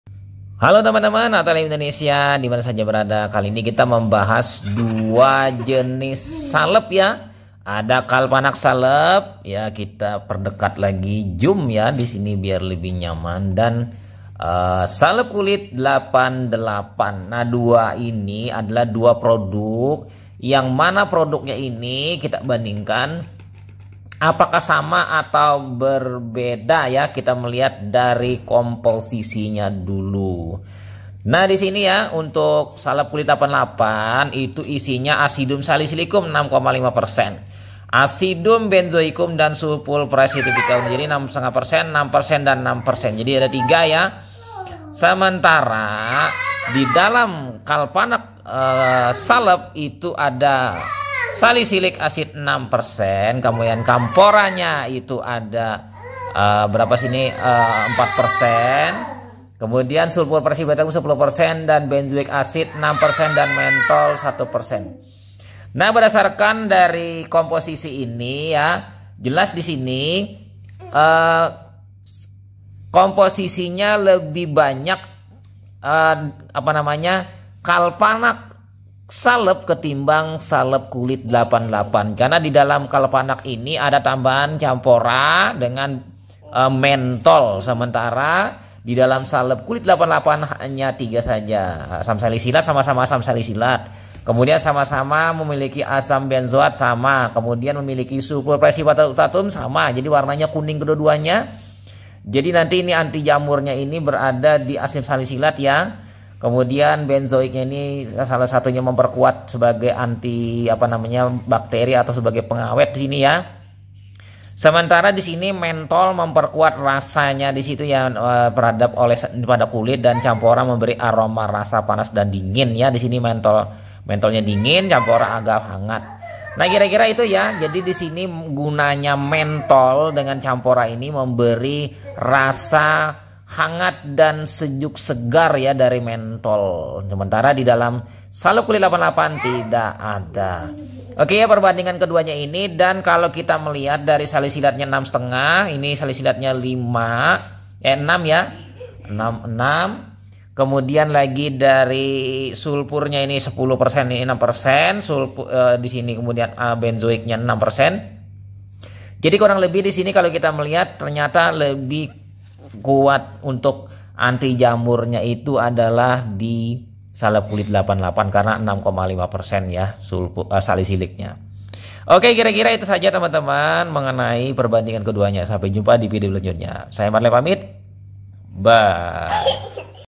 Perbandingan Salep Kulit 88 dan Kalpanax Salep | antijamur | Panu kadas kurap kutu air
Link Pembelian Kalpanax Salep